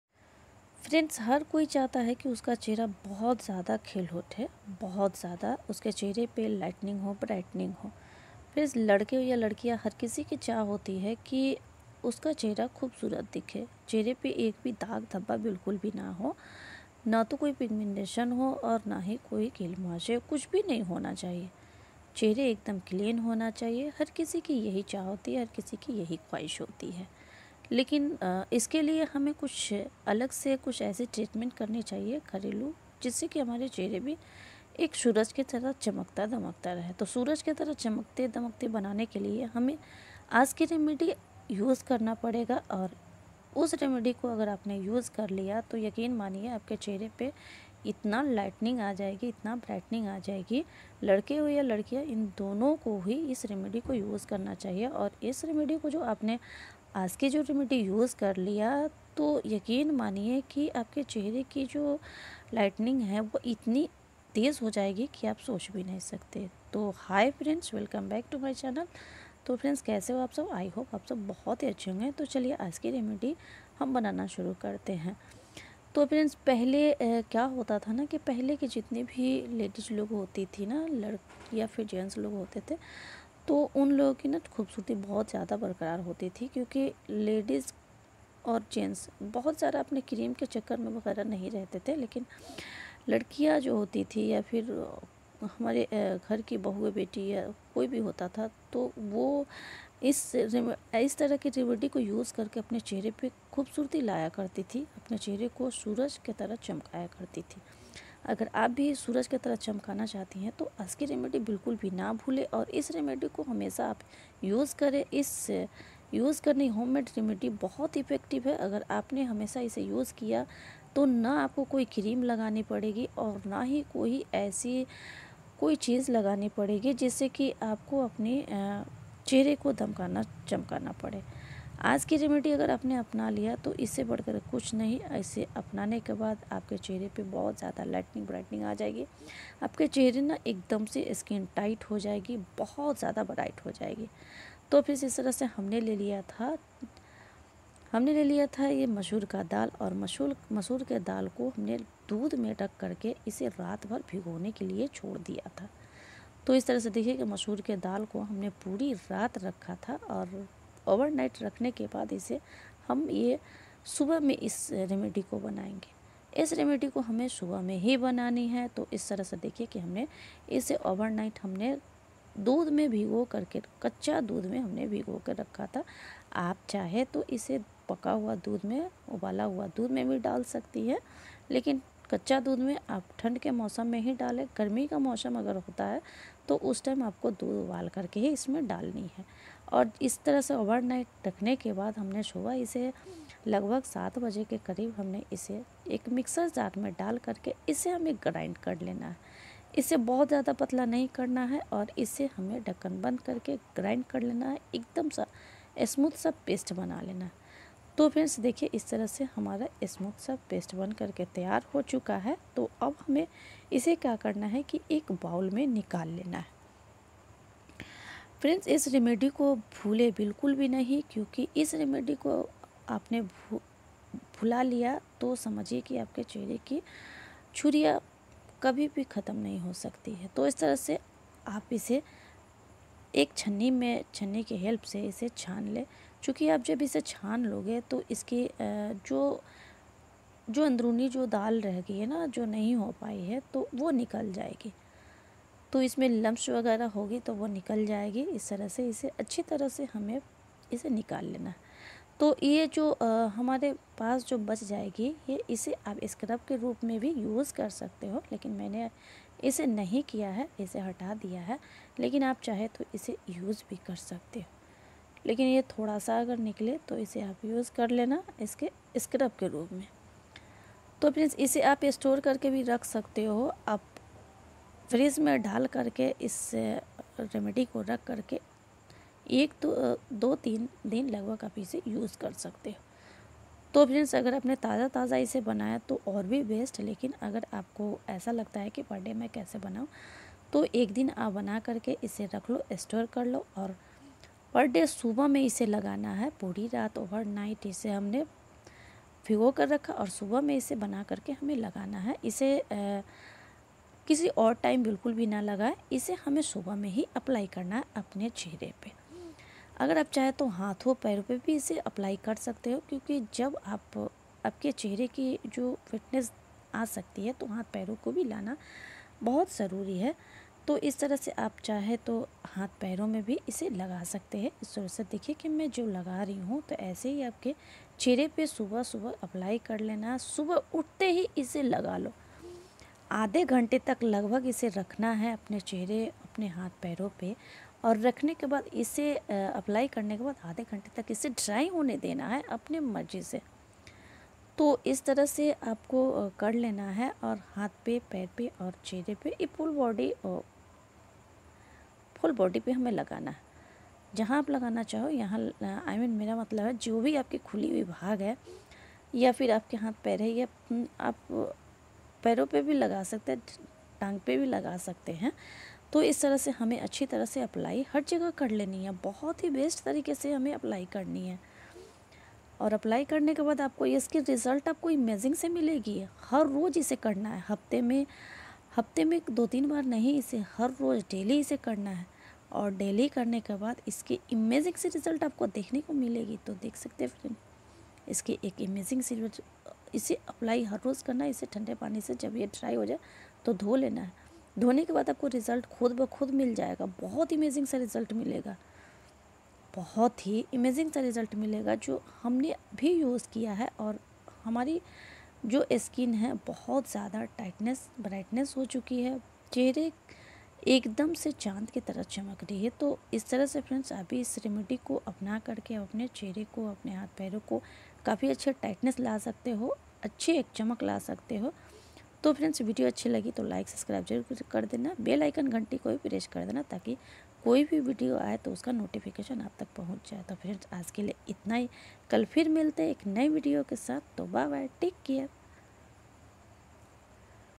Glowing Skin Home Remedy| Glowing Skin Tips|Skin Care Tips
सुबह उठते ही बस इसे लगालो चेहरा सूरज सा चमकेगा|ढीला स्किन टाइट कर जवान बना देगा येGlowing Skin Home Remedy| Glowing Skin Tips|Skin Care Tips